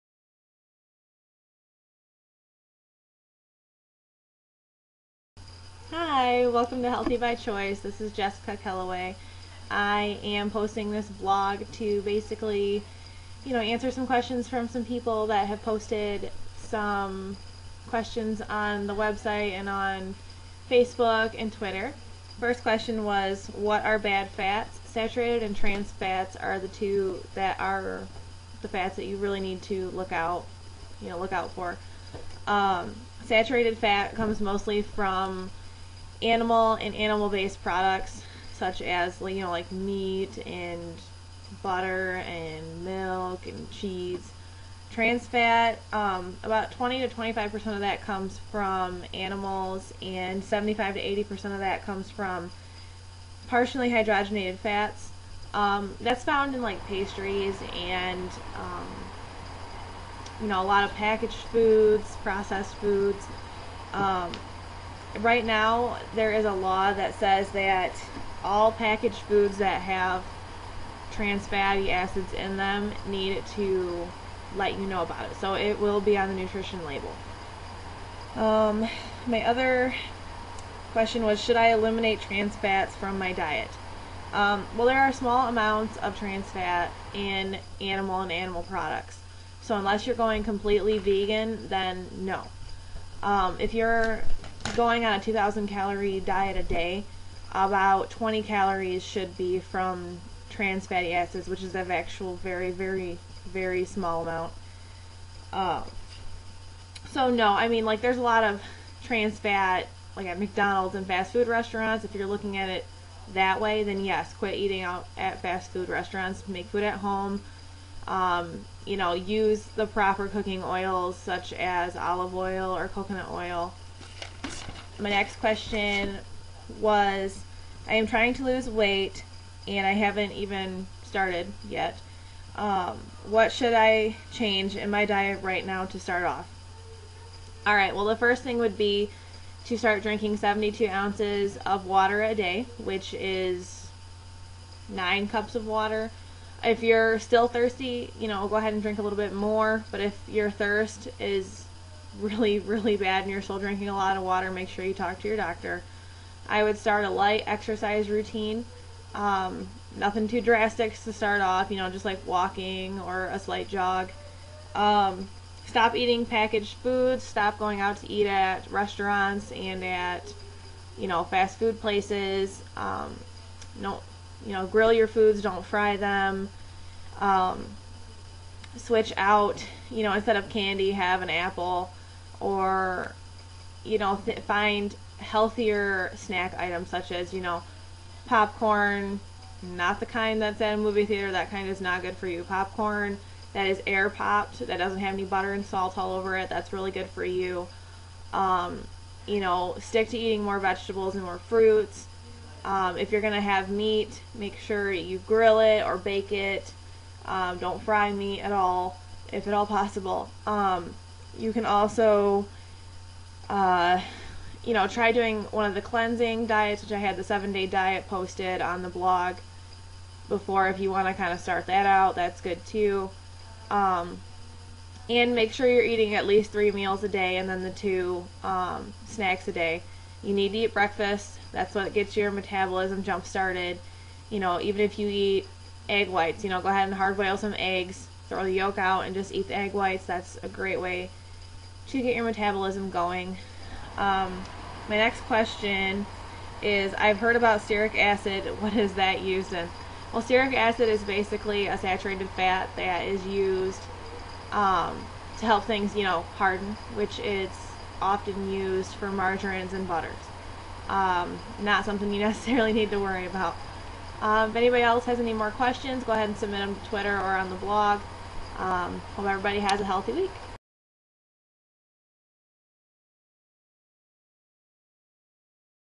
Q&A Session- Fats
Welcome! This video touches base on bad fats, transfat elimination, and beginning a diet!
We only got a few questions this week! Please submit more!